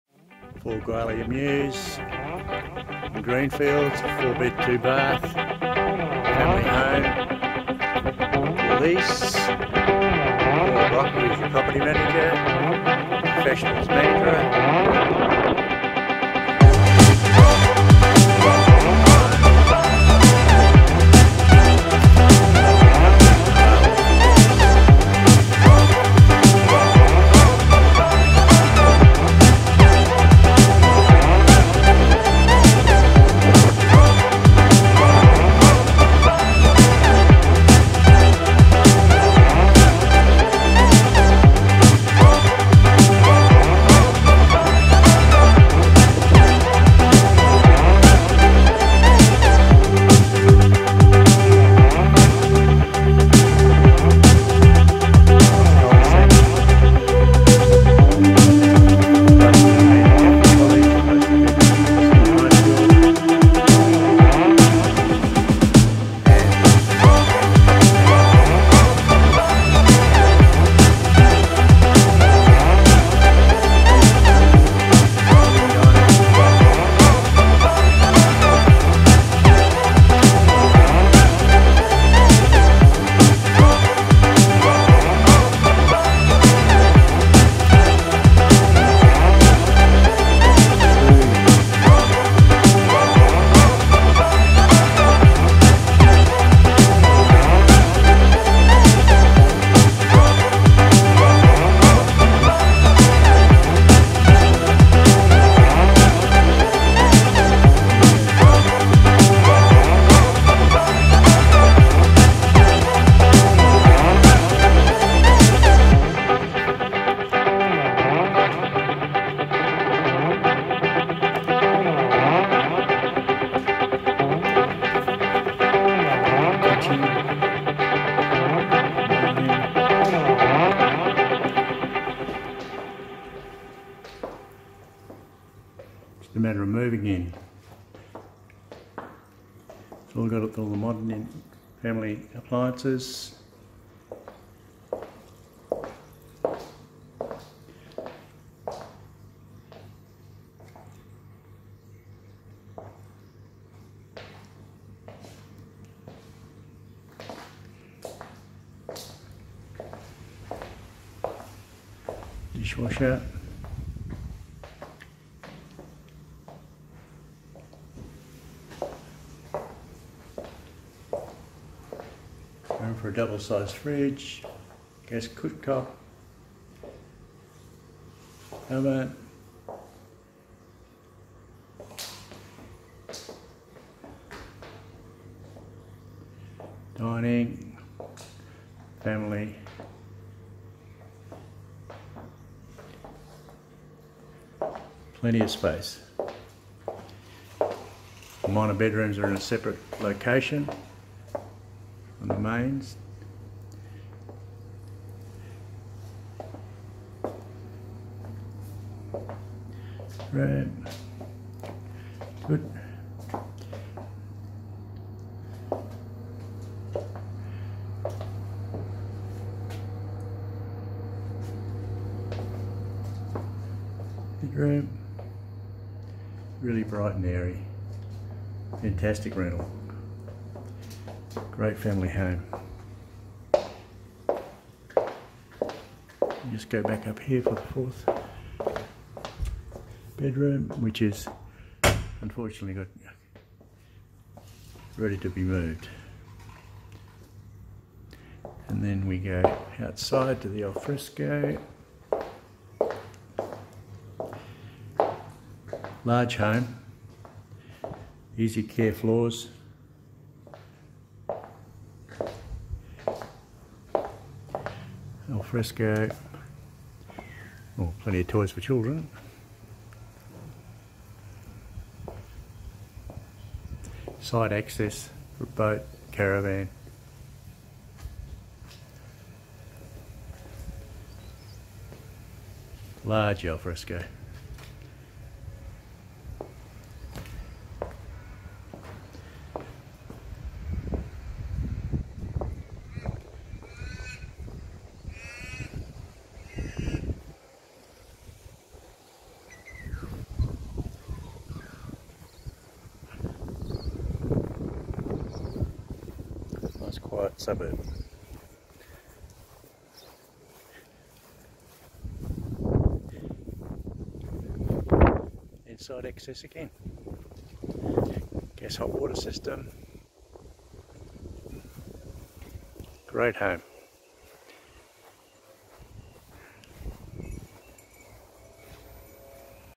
For Lease 4 Gwallia Mews , Greenfields
Four Bedroom Two Bathroom Home For Lease Close to Mandurah Catholic College and Coodanup Schools. Medical, Dental and Major Shopping Centre